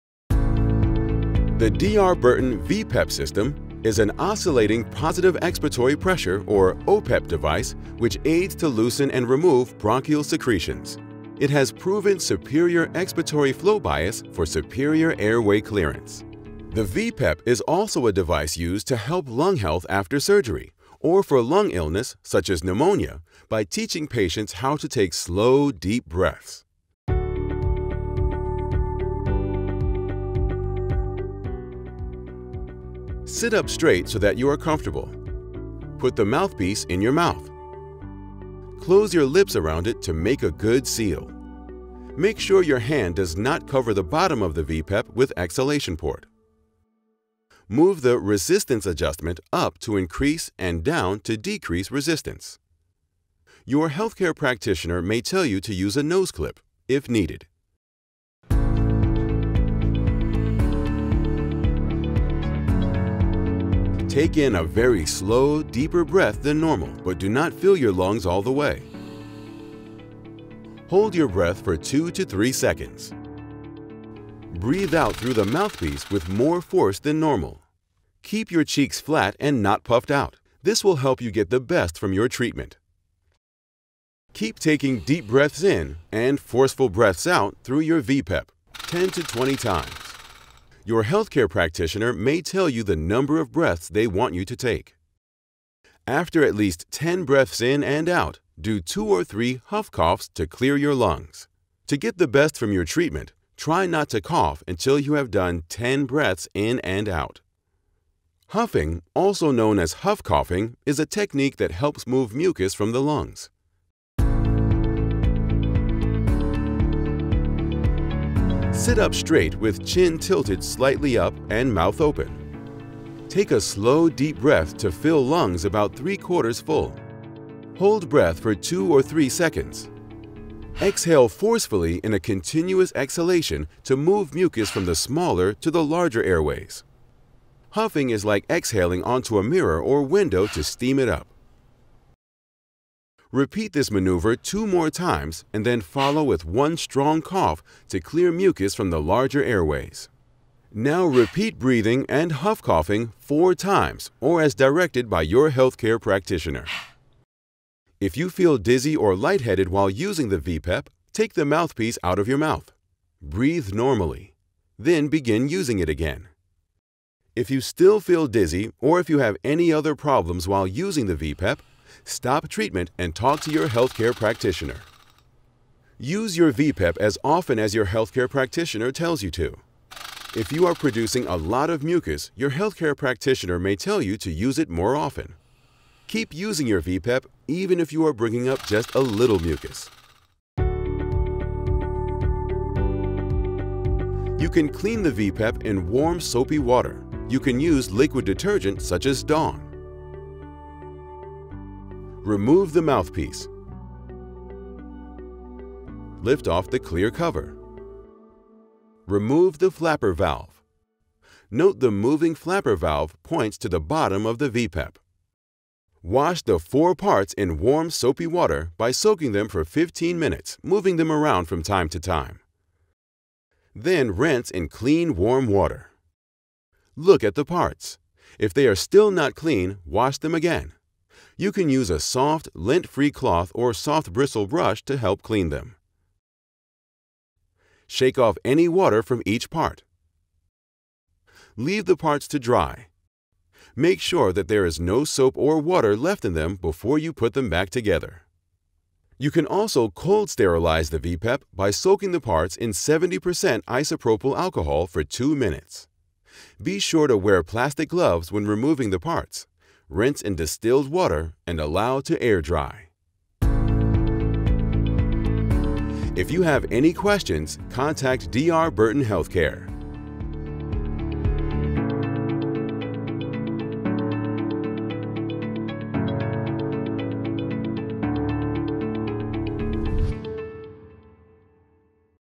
D R Burton vPEP video
Instructional video on the vPEP oscillating positive expiratory pressure (OPEP) system